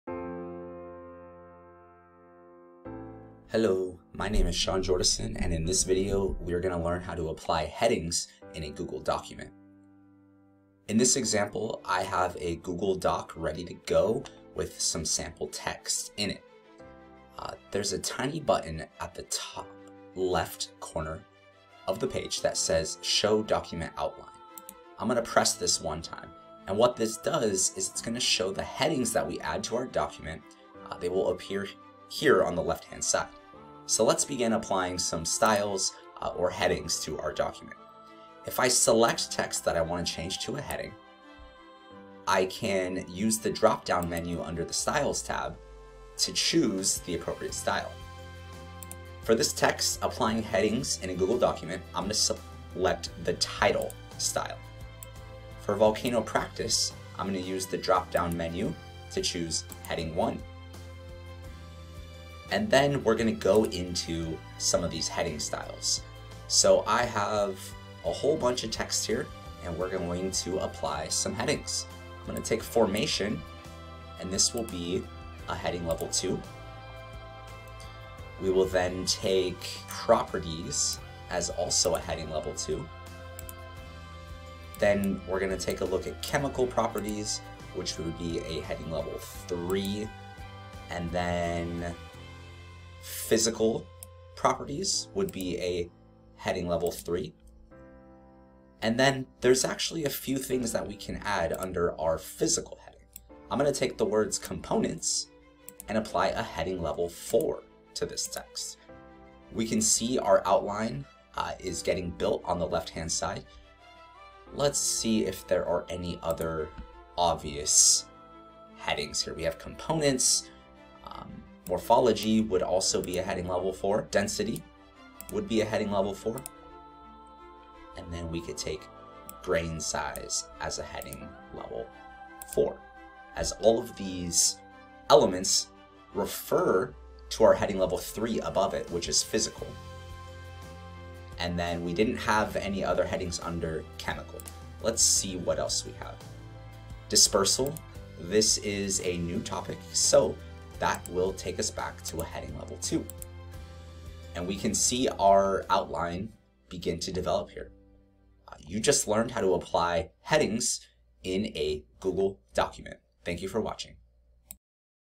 Apply Headings to Google Docs
This video will show how to apply accessible styles and headings to a google doc.